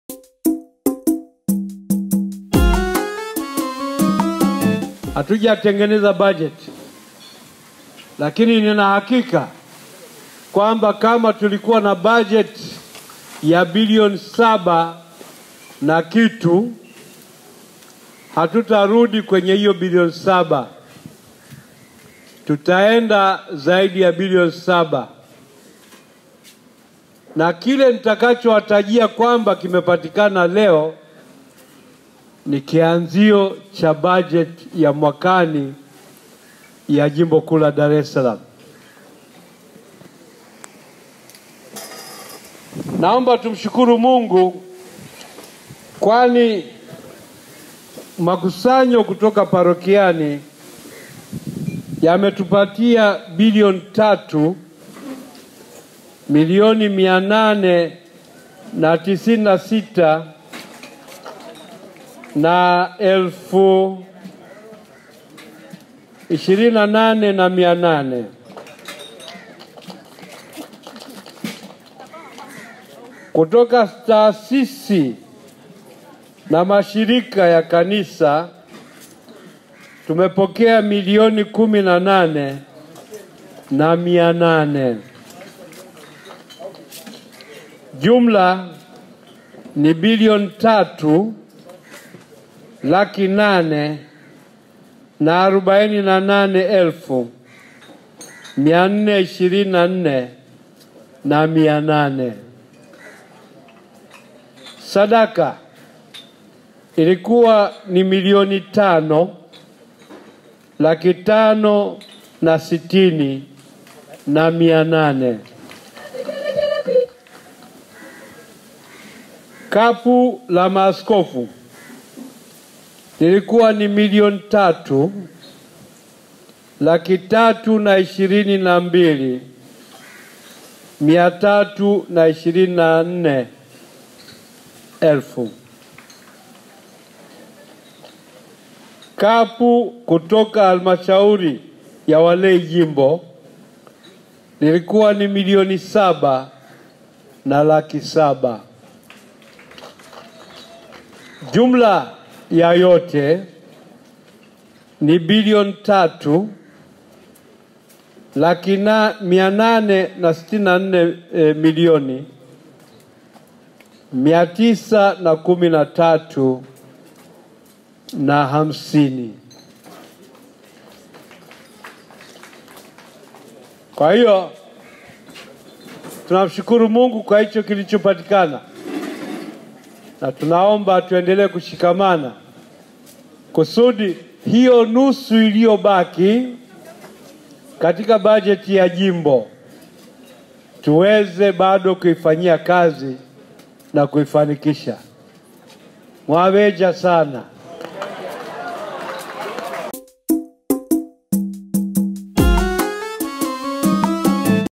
Matokeo ya kilichopatikana kwenye Mavuno Jimbo kuu Katoliki la Dar es salaam 2022 yatangazwa..
ASKOFU MKUU JUDE THADDAEUS RUWA'ICHI AKITANGAZA KILE KILICHOPATIKANA KATIKA MAVUNO JIMBO 2022 | Picha hizi zinatengenezwa na TUMAINI MEDIA. Matumizi yoyote ya picha hizi  ni marufuku bila idhini kutoka TUMAINI MEDIA